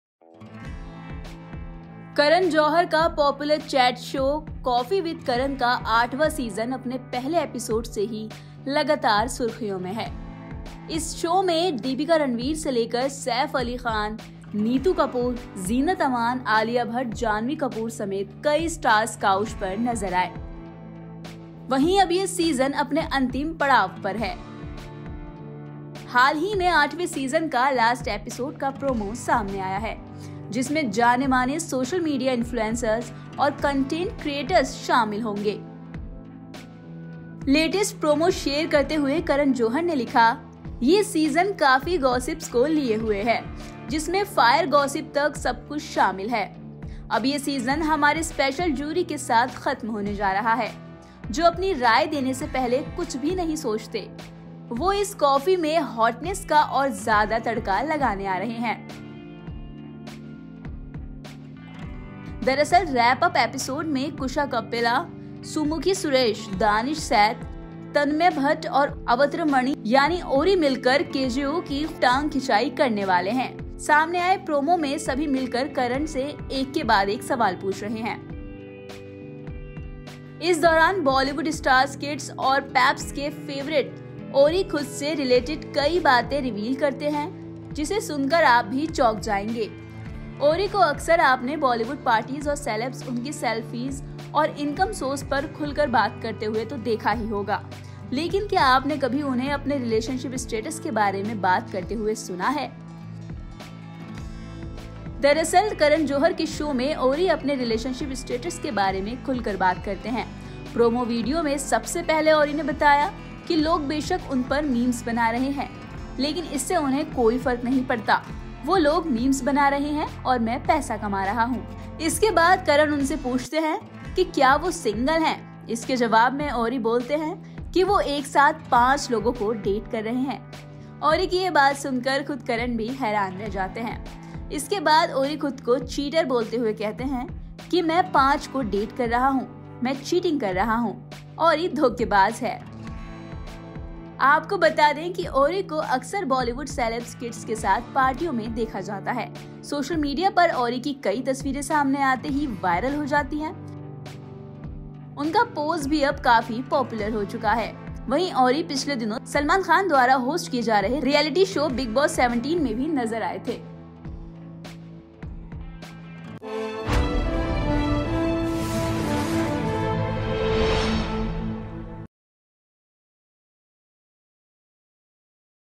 एक साथ कर रहे हैं Orry 5 लोगों को डेट, Coffee with Karan में किया खुलासा
करण जौहर का पॉपुलर चैट शो 'कॉफी विद करण' का आठवां सीजन अपने पहले एपिसोड से ही लगातार सुर्खियों में है। इस शो में दीपिका-रणवीर से लेकर सैफ अली खान, नीतू कपूर, जीनत अमान, आलिया भट्ट, जान्हवी कपूर समेत कई स्टार्स काउच पर नजर आए। वहीं अब यह सीजन अपने अंतिम पड़ाव पर पहुंच गया है।